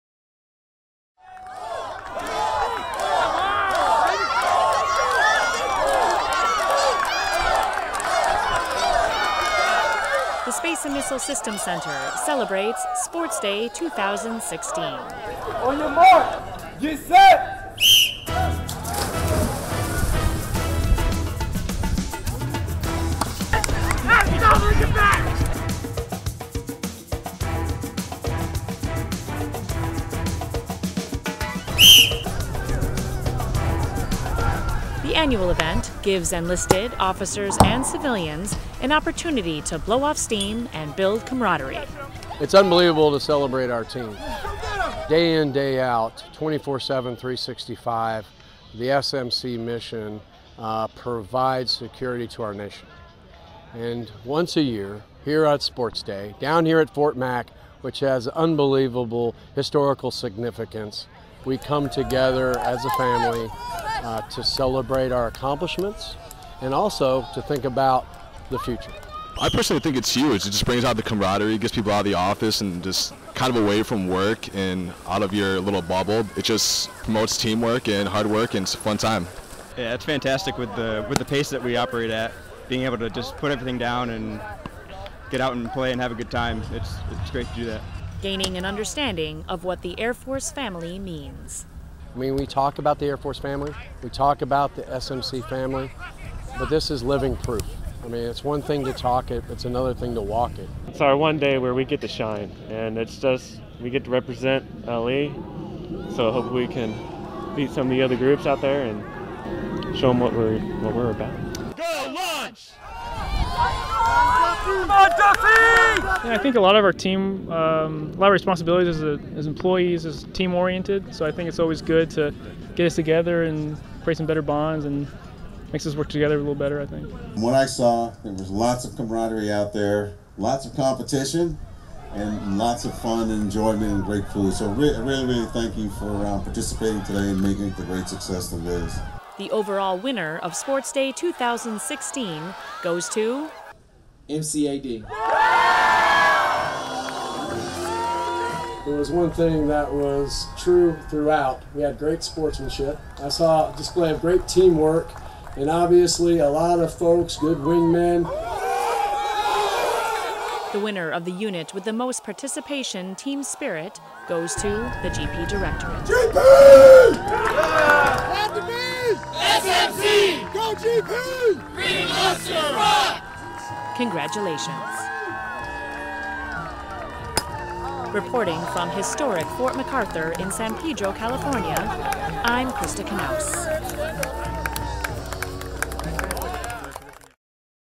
SMC Sports Day 2016
The Space & Missile Systems Center celebrates Sports Day 2016 at Fort MacArthur in San Pedro, California.